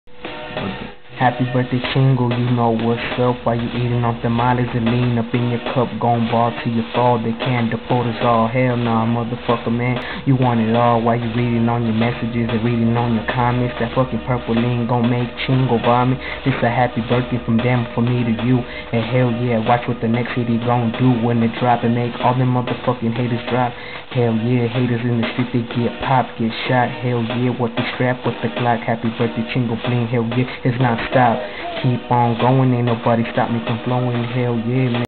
My brother again freestylein for CHINGO BLINGS Birthday
Dont talk shit! my brother sent this video to chingo bling and he put it on his videos.